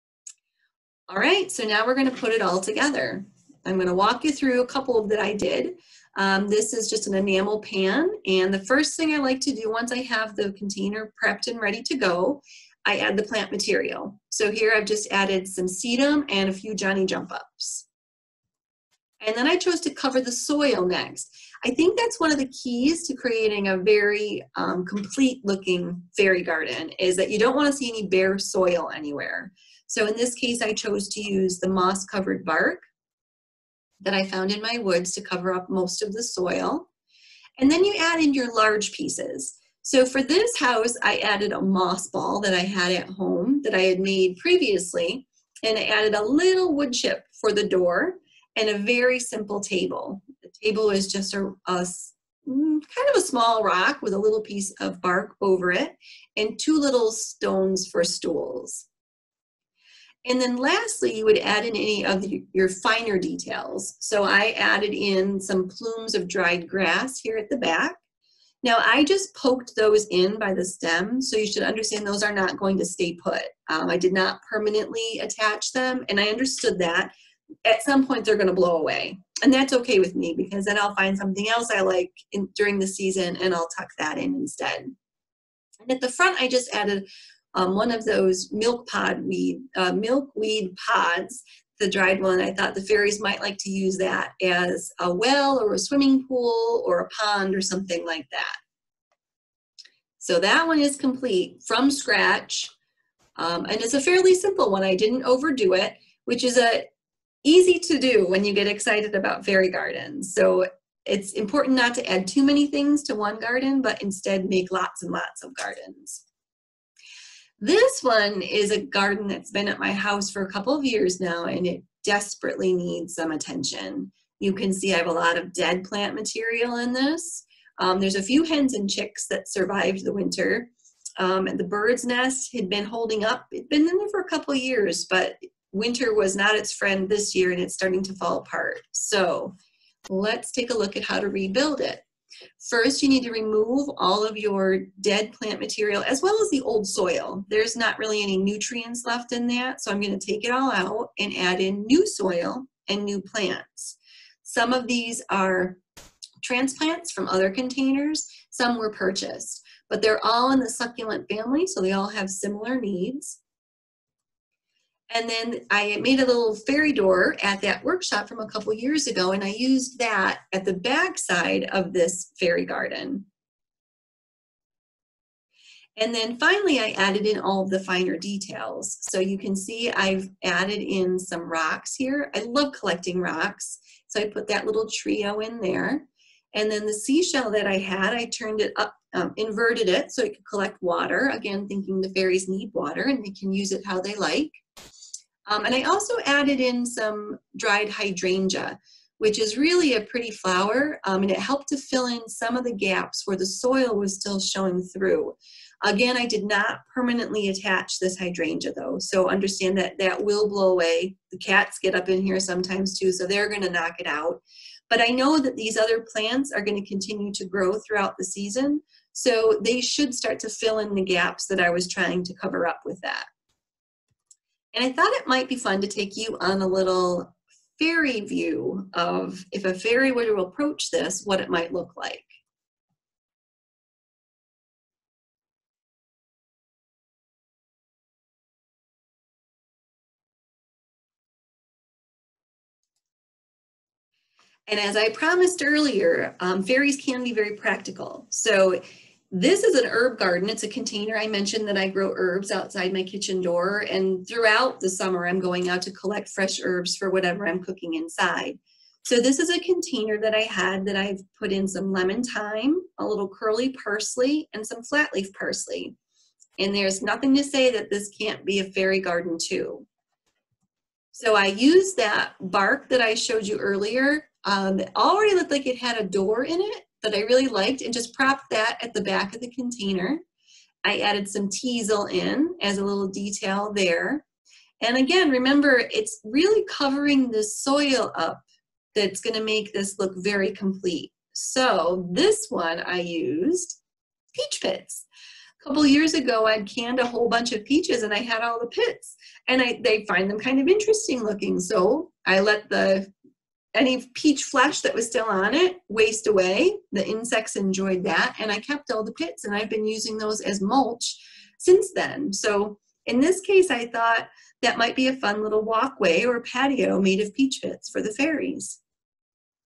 Fairy Gardens- Putting It All Together-5
Watch as Renee creates 3 unique Fairy Gardens and take a Fairies' eye tour of one too!